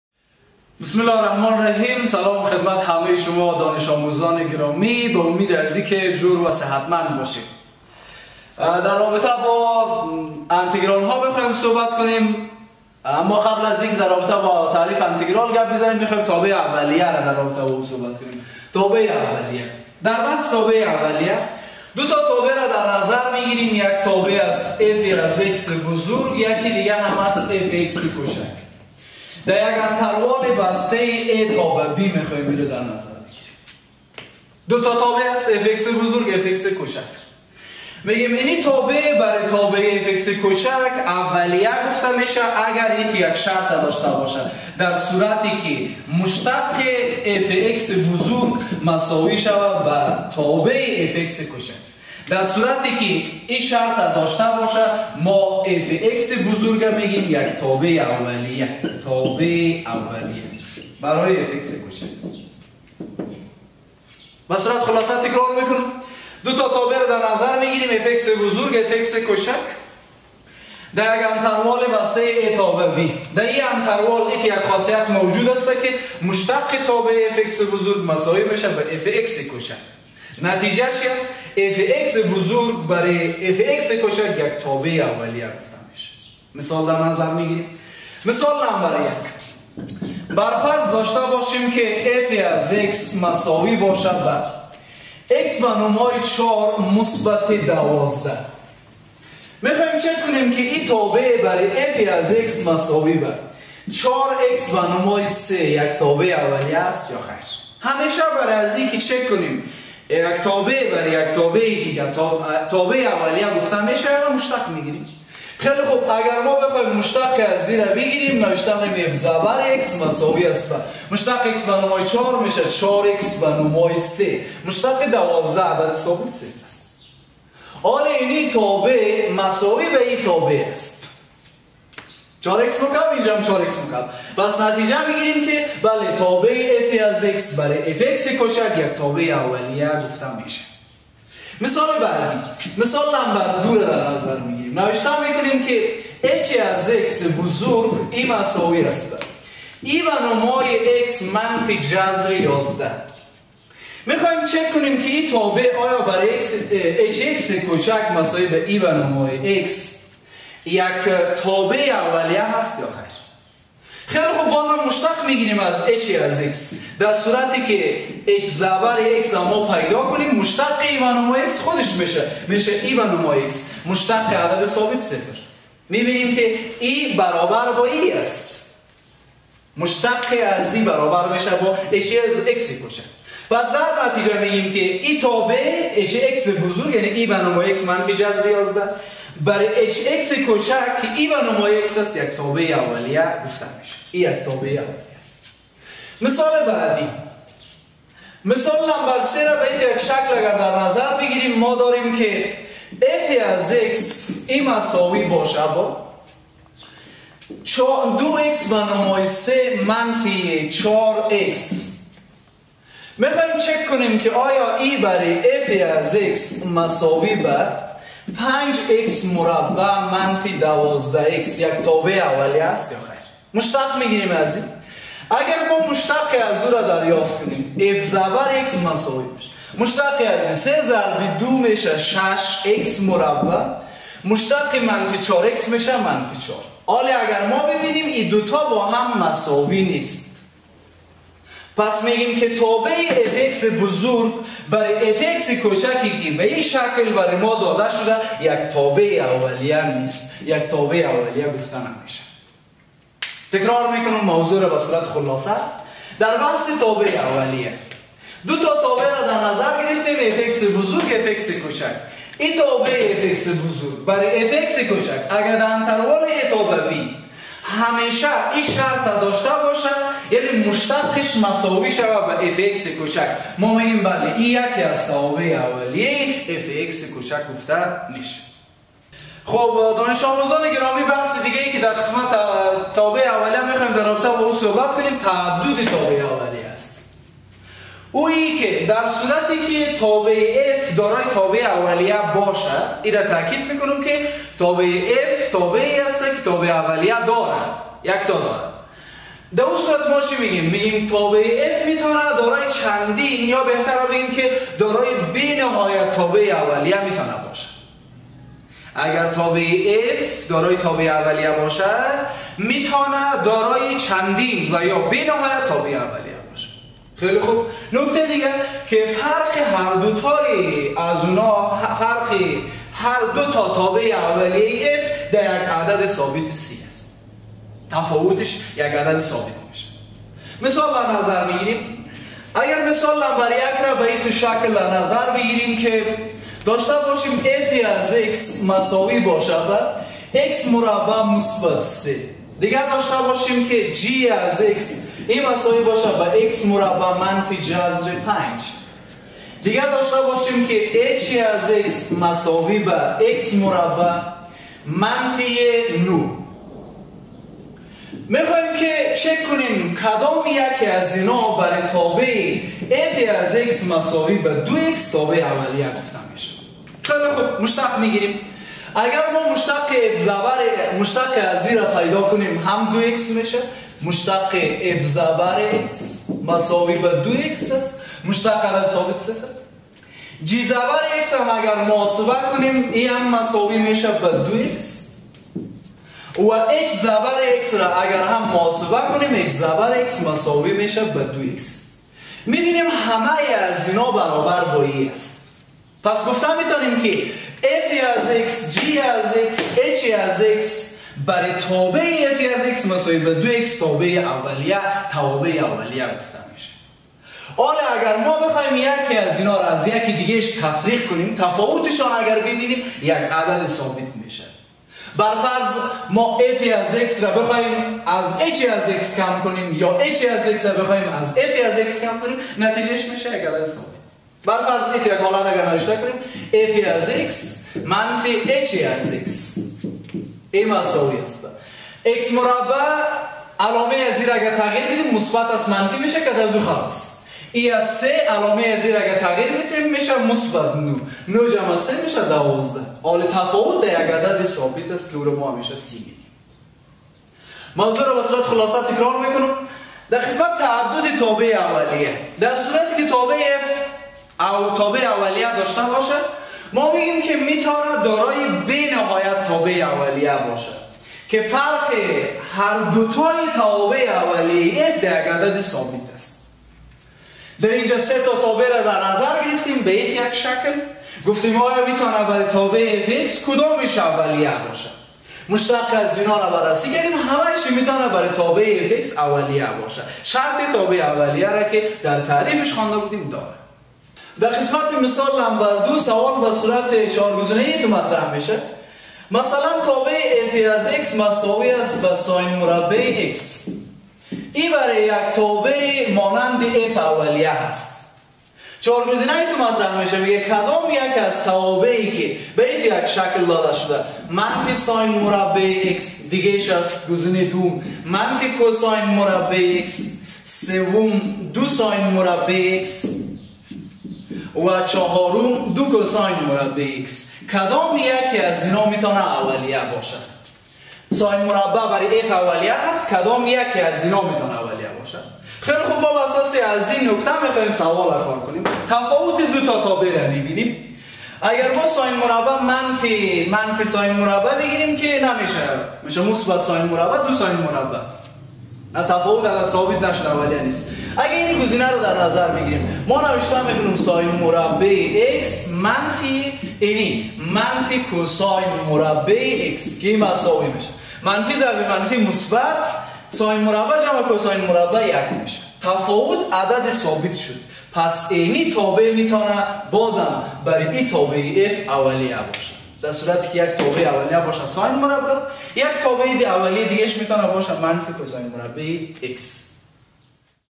ریاضیات عالی انتگیرال‌ها درس اول - تابع اولیه (ریاضیات عالی - درس هفدهم)
ویژه دانش آموزان کانکور و شاگردان مکاتب.
ریاضیات عالی انتگیرال‌ها
همراه با حل سوالات فورم کانکور
استاد : انجنیر نجیب الله نجیب